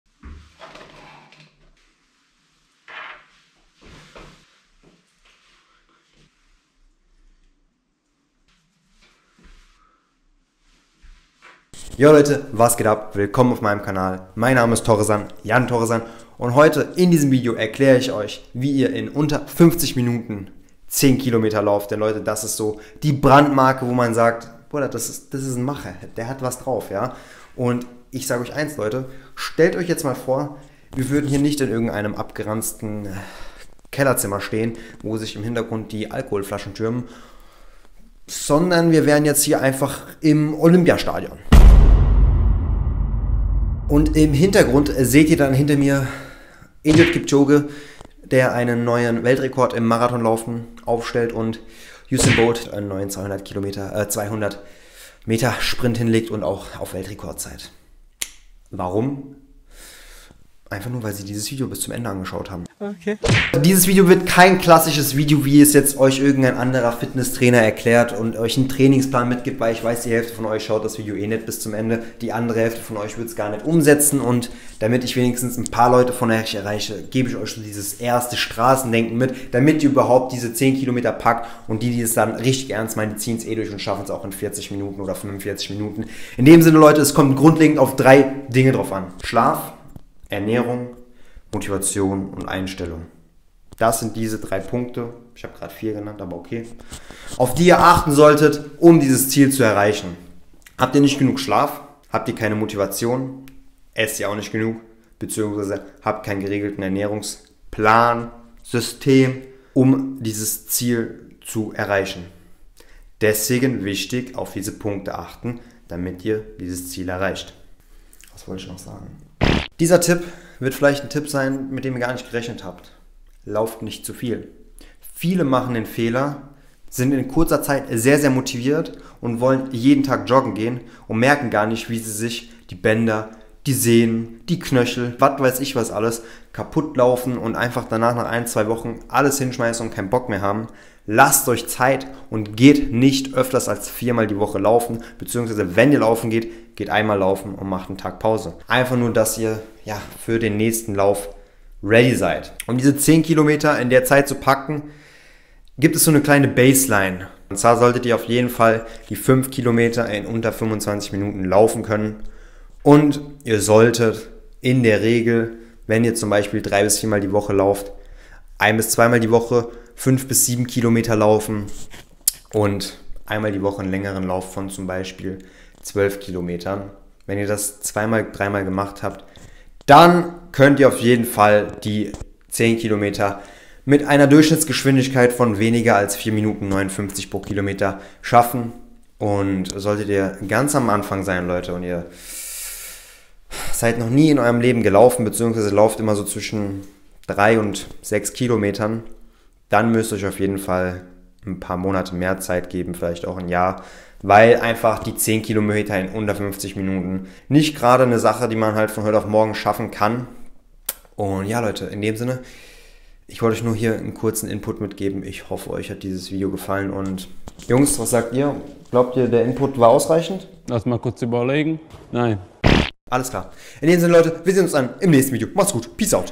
10 km in unter 50 min | LAUFTIPPS
In diesem Video erklär ich euch, basierend auf 3 grundlegenden Bausteinen, wie IHR 10 km in unter 50 min laufen könnt.

Ich bin mir sicher, dass noch einige weitere Punkte gibt, die man hätte nennen können... jedoch wollte ich euch "meine" wichtigsten Tipps mit auf den WEG geben und das Video nicht zu lang machen.